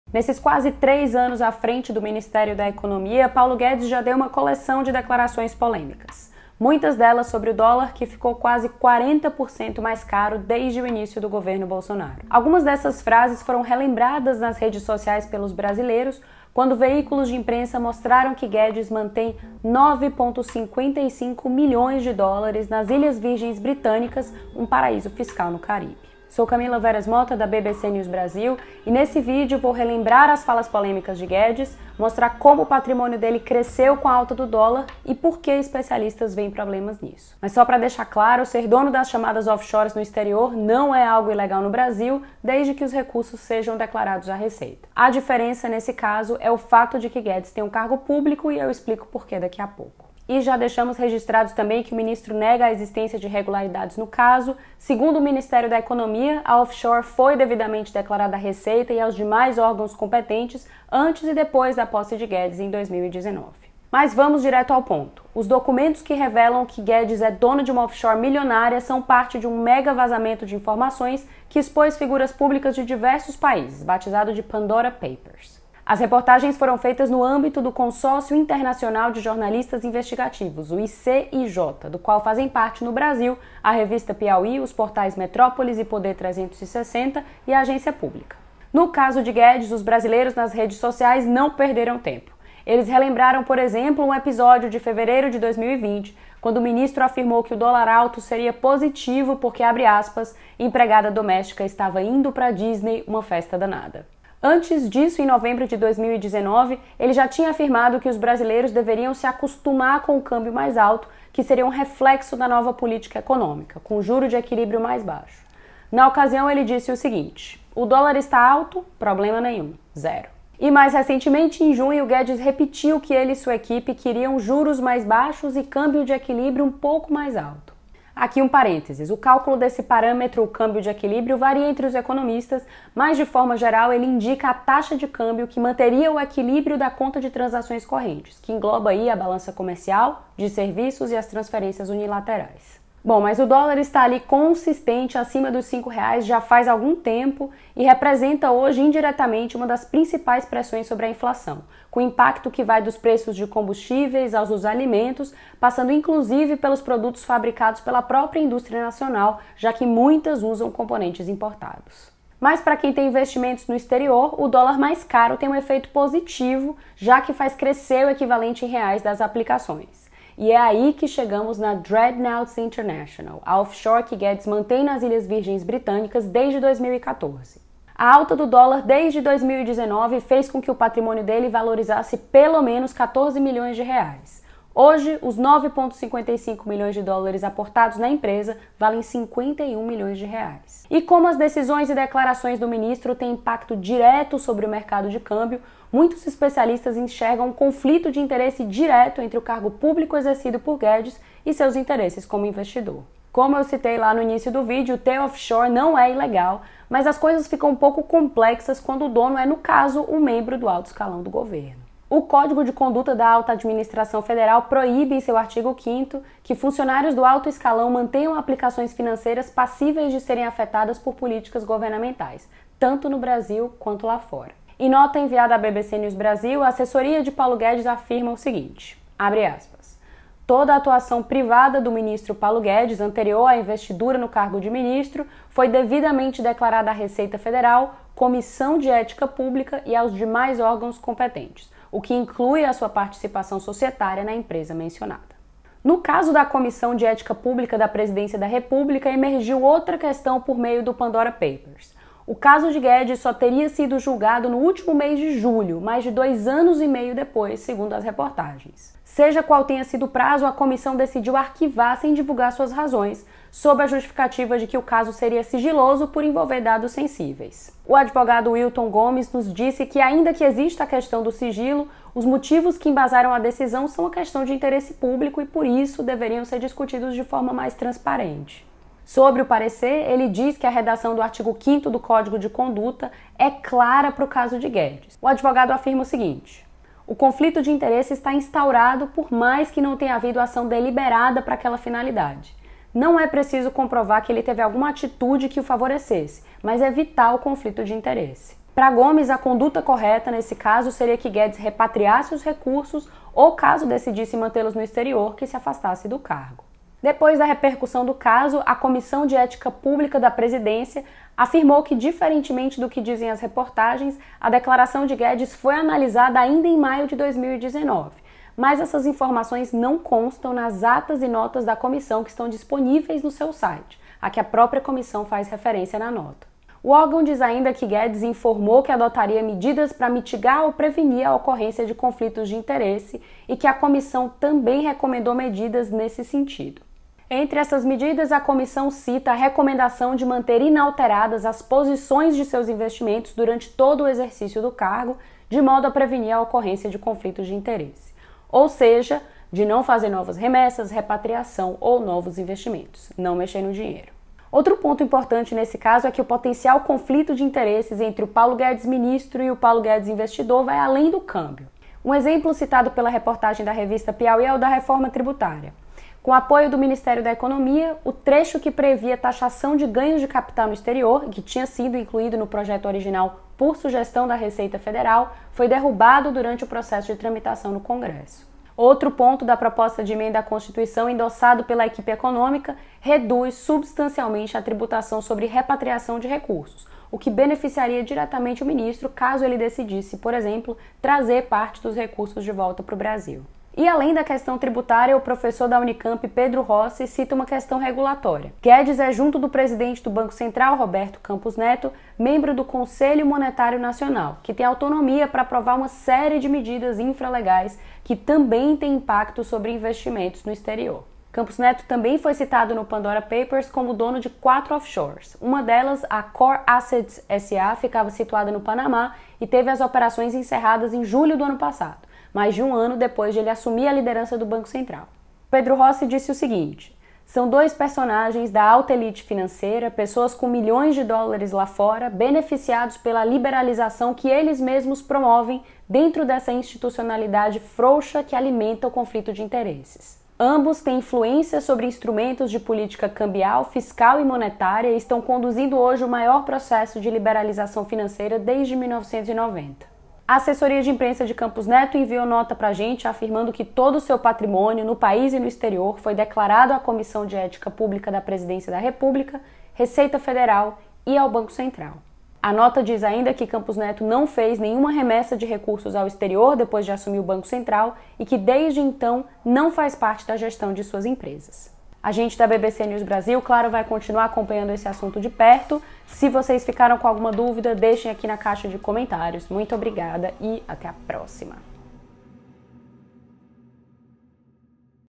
Como alta do dólar elevou fortuna de Guedes em paraíso fiscal no exterior
Em quase três anos à frente do ministério da Economia, Paulo Guedes deu uma coleção de declarações polêmicas – muitas delas envolvendo o dólar, que ficou quase 40% mais caro desde o início do governo Bolsonaro.

Algumas dessas frases foram relembradas nas redes sociais pelos brasileiros quando veículos de imprensa mostraram em reportagens do megavazamento Pandora papers que Guedes mantém US$ 9,55 milhões nas Ilhas Virgens Britânicas, um paraíso fiscal no Caribe.

Neste vídeo, Camilla Veras Mota lembra que o ministro, que em fevereiro de 2020 causou polêmica ao afirmou que o dólar alto seria positivo porque “empregada doméstica estava indo para a Disney, uma festa danada”, viu seu patrimônio em reais crescer com a alta do dólar.

Guedes nega irregularidades e diz que os órgãos competentes foram avisados sobre seu patrimônio antes de sua posse no Ministério da Economia.

Ter offshores não é crime no Brasil – mas a situação fica complexa no caso de um servidor público do alto escalão do governo.

Confira.